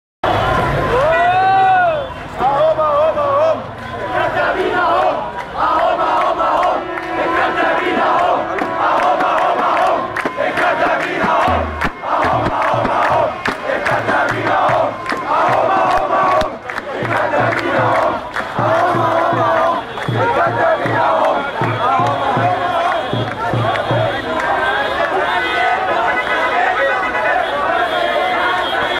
Protesters denouncing State TV المتظاهرون ينددون بالتلفزيون المصري
المتظاهرون أمام مبنى التلفزيون المصري في ماسبيرو ينددون بالكذب والتحريض ضد الثوار، بعد مسيرة من التحرير يوم ٢٨ أكتوبر